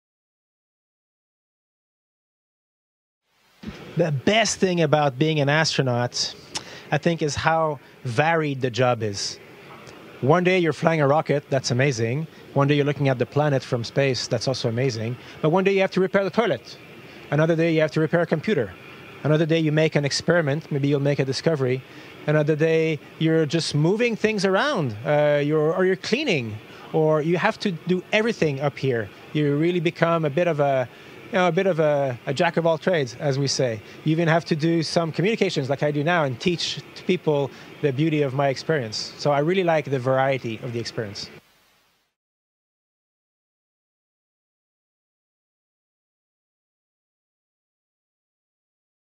Astronaut - Questions and answers with David Saint-Jacques live from space
During the We Day Montreal event, Canadian Space Agency astronaut David Saint-Jacques answers the following question, live from the International Space Station: What is the best thing about being an astronaut? (Credits: Canadian Space Agency, NASA)